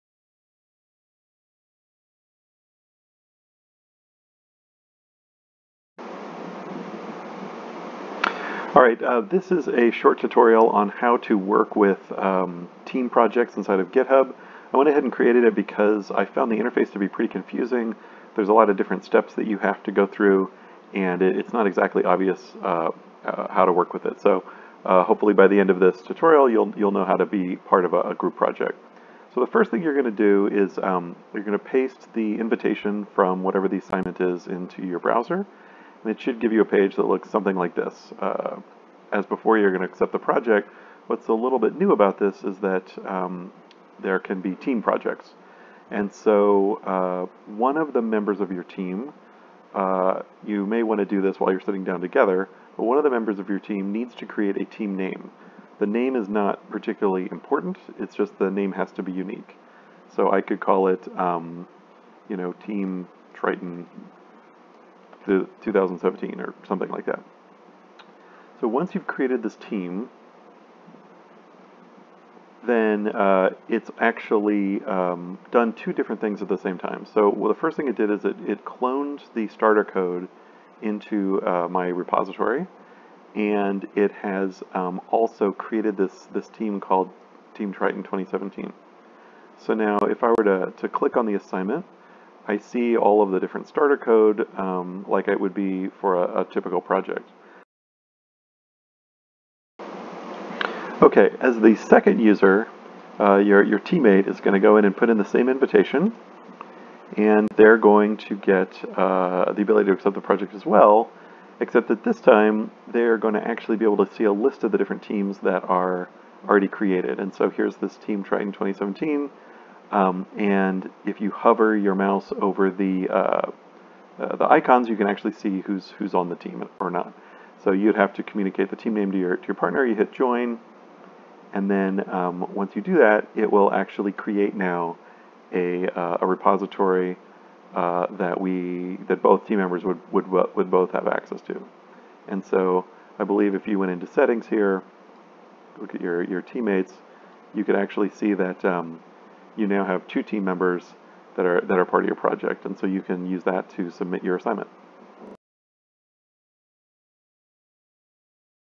Working with GitHub Classroom teams
A short description of joining a team project as part of GitHub classroom.